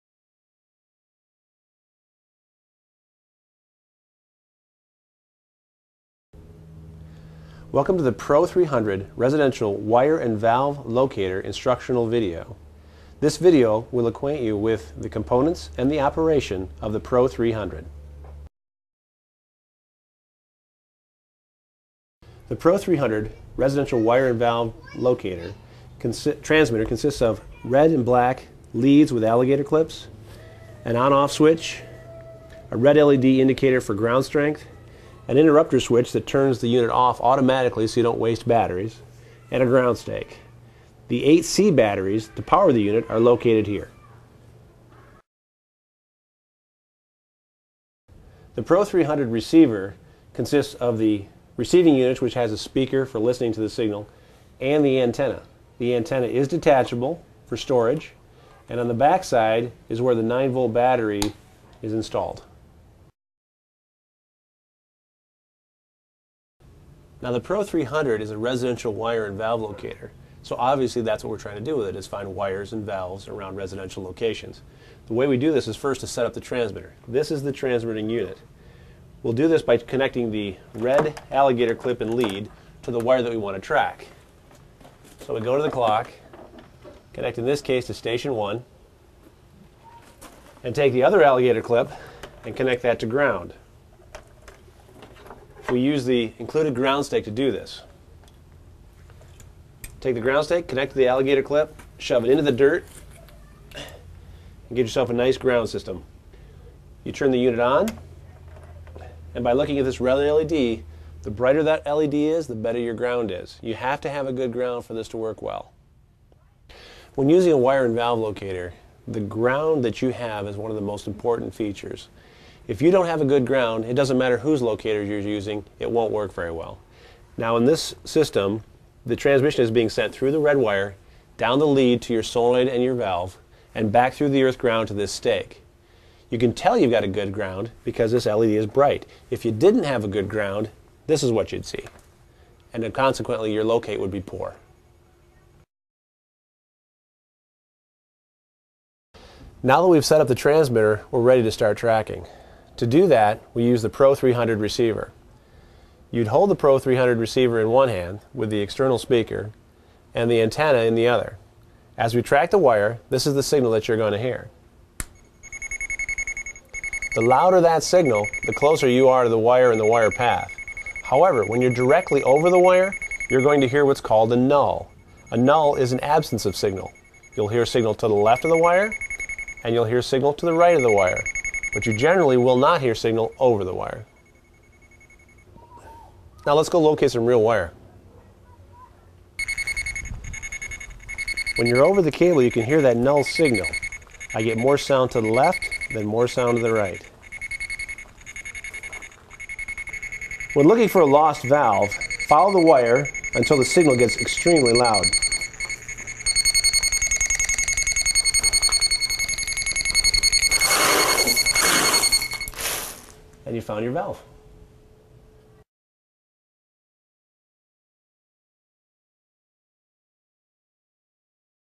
How to use an Armada Technologies Pro300 Sprinkler Valve or Solenoid Locator Instructional Guide
The Pro300 Sprinkler Wire and Valve Locator is designed to track irrigation wires and find lost sprinkler solenoids in home irrigation systems.  This 3 minute video describes the functions and shows how to use the Pro300.